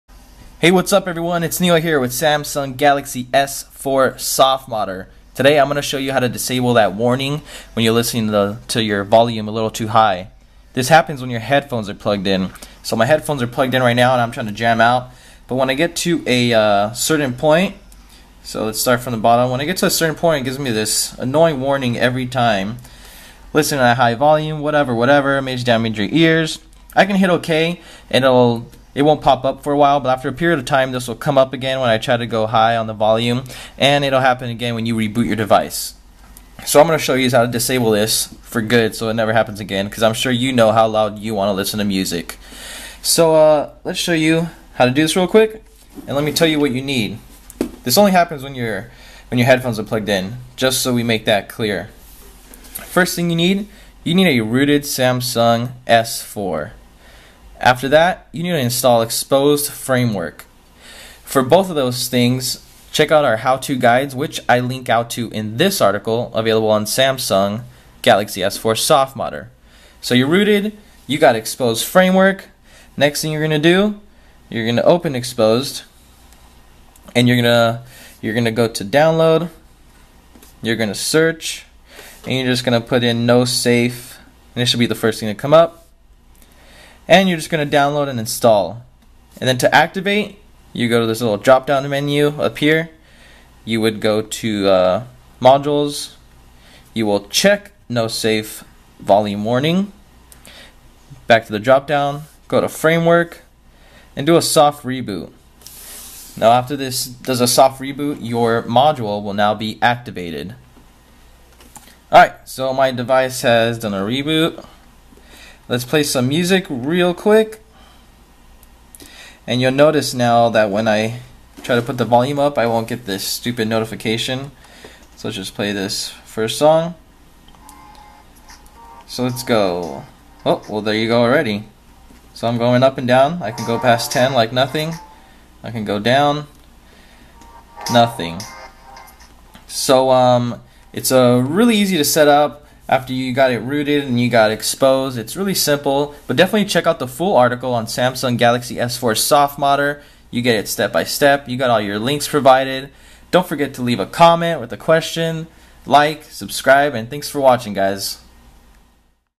Disable the High Volume Warning When Listening to Music with Headphones - Samsung Galaxy S4 [How-To]
How to Disable the High Volume Alert [GS4]

In this softModder tutorial, I'll be showing you how to disable that annoying high volume warning that pops up when you have your headphones plugged in and raise the volume too high. It's as easy as installing a mod on your rooted GS4.

This mod in question is called NoSafeVolumeWarning, and you'll need to be rooted with Xposed installed to use it. You could also use Wanam Xposed if you want. Use our previous guide to learn how:

For more information and download links, check out the "full tutorial" over on Samsung Galaxy S4 using the link up top.